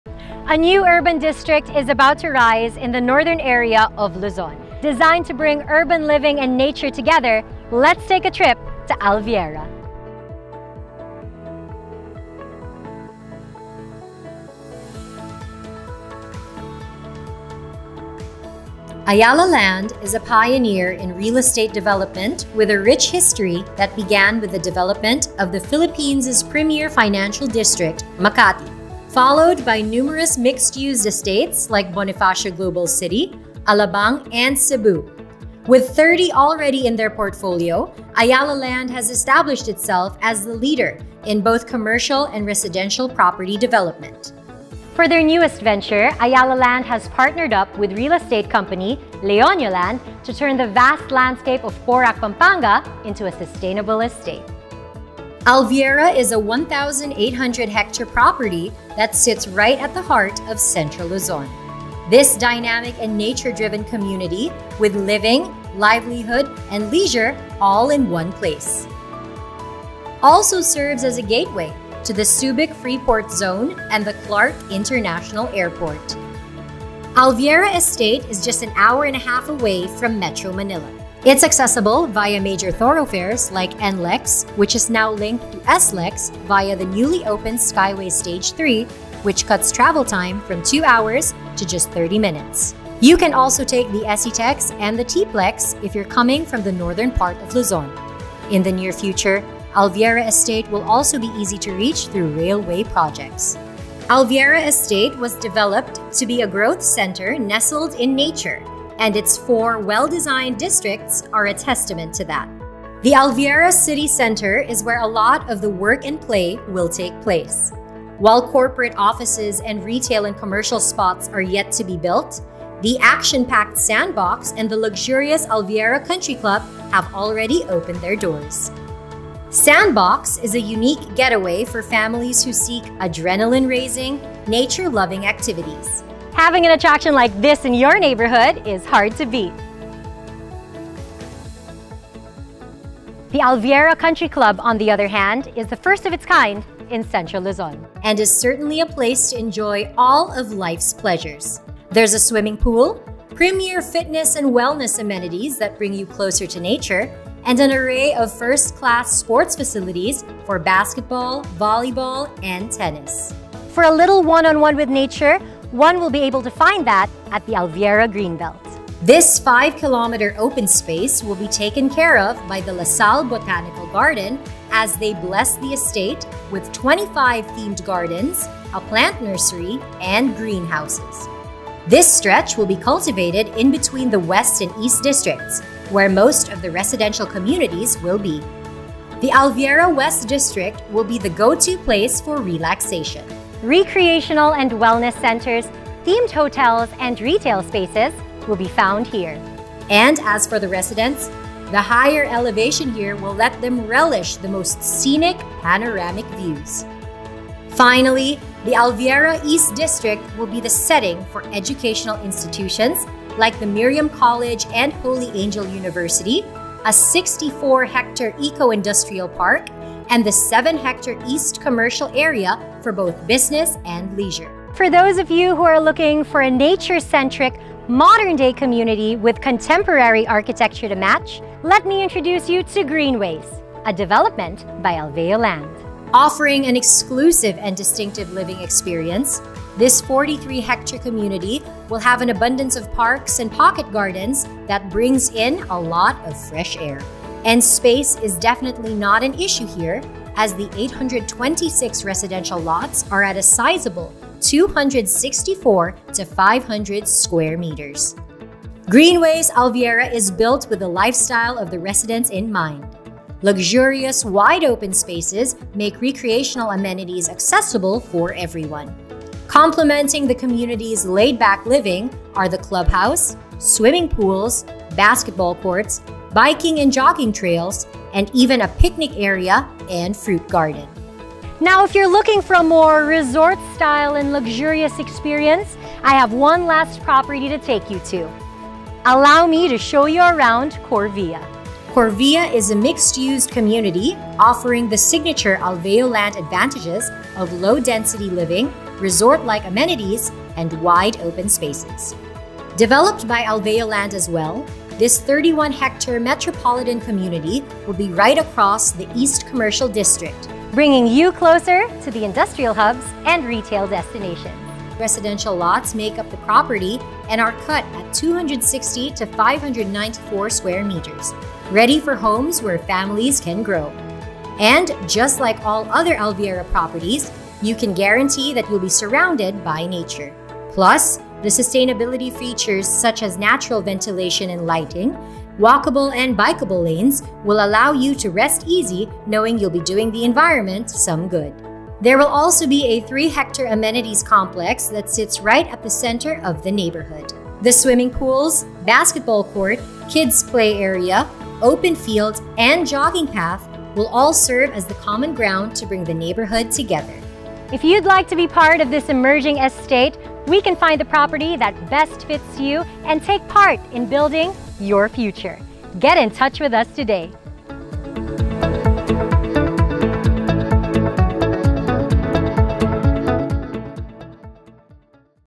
Alveo in Alviera
Discover Alveo Land's latest residential portfolio in Ayala Land's emerging growth district in Alviera, Pampanga.

Corvia is 31-ha contemporary community located in Alviera East. As Alveo Land's third residential community in Alviera, it draws together commercial, leisure, and lifestyle environments into a vibrant core. Corvia is the only residential village that will be directly across Alviera East Commercial district, and closest to Holy Angel University.

The Greenways is a 43-hectare contemporary community where lifestyles flourish, bringing senses to a heightened experience of every day, with green open spaces within and around. It offers complete amenities and facilities for active and laid-back pursuits, revitalizing the living experience amidst an invigorating environment.